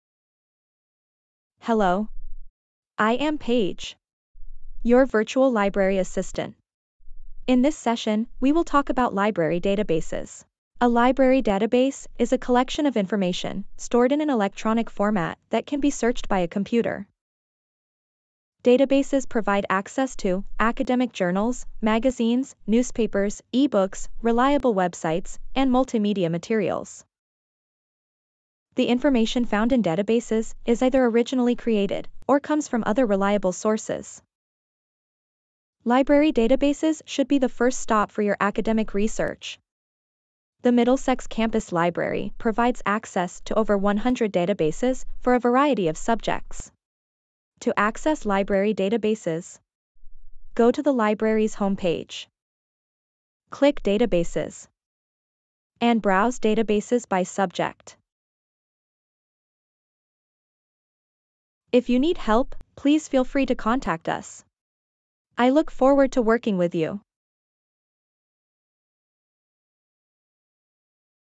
CT State Middlesex Campus Library Databases
This video talks about Middlesex library databases.